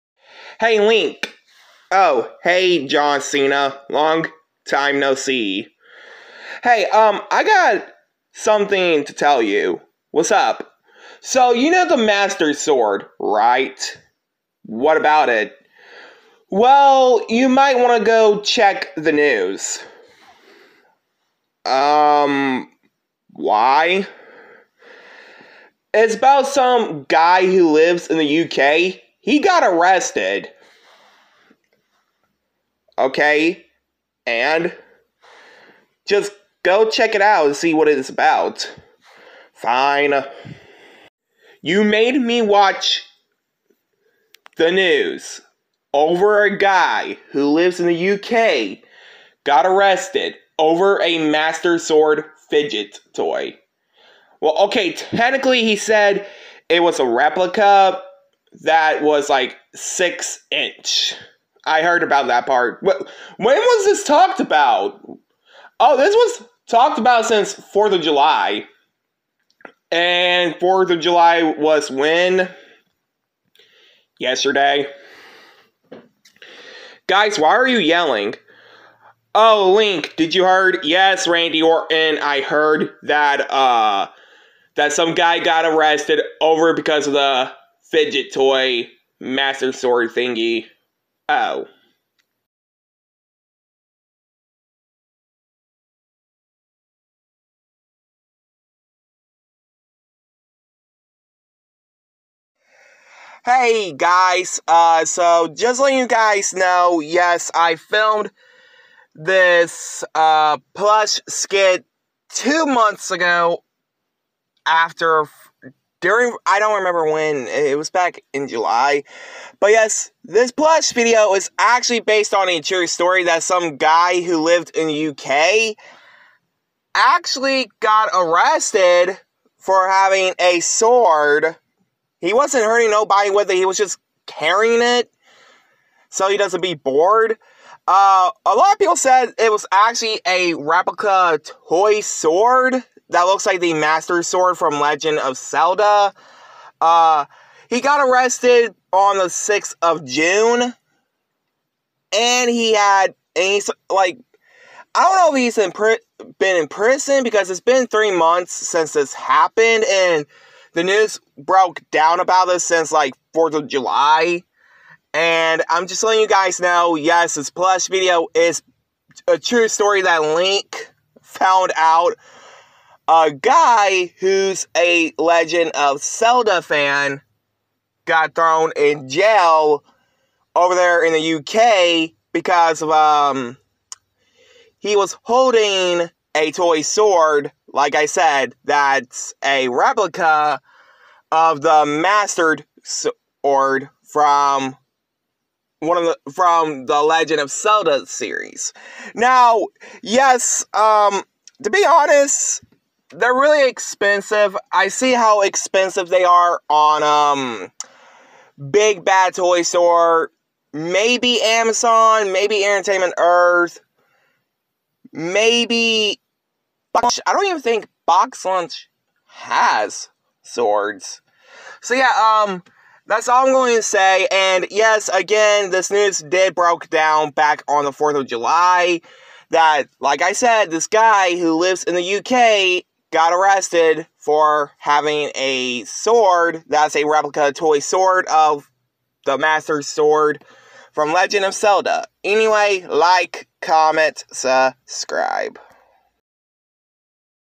John Cena & The Angry Birds Season 8 Episode 54:Link Finds Out!
In This Episode:
Link Finds Out A Man In The UK Got Arrested
(New Plush Skit On September 24th & 27th)